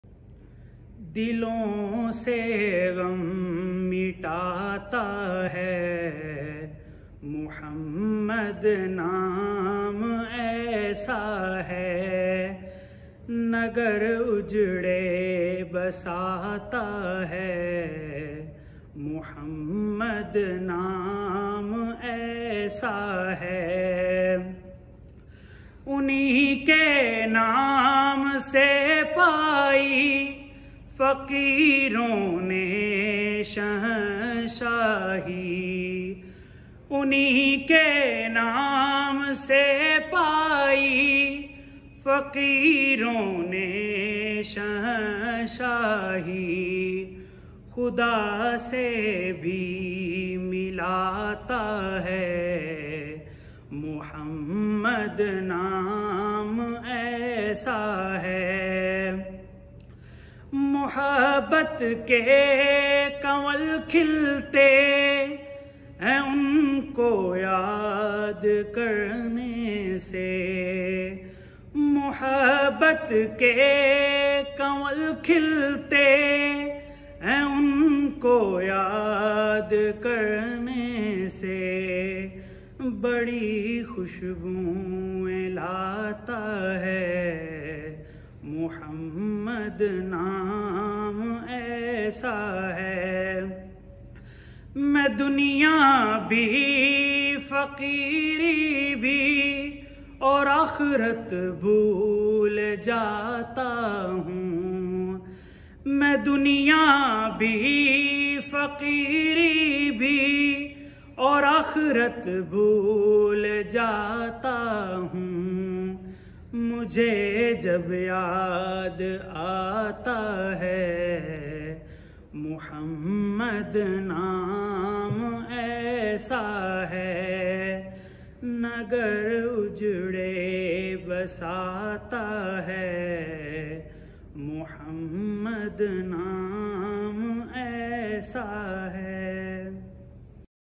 MSO Olympiad ‘22 Qiraat and Naat competition. RAHBAR MEDICAL AND DENTAL COLLEGE LAHORE
MSO Olympiad ‘22 Qiraat and Naat competition.

Match 16

Vote for SHOAIB MAQBOOL   in comments
RAHBAR MEDICAL AND DENTAL COLLEGE LAHORE

 Registeration link for MSO Olympiad 22 Qiraat and Naat Category
Last date of registration 25th of August
Last date of Submission 25th of August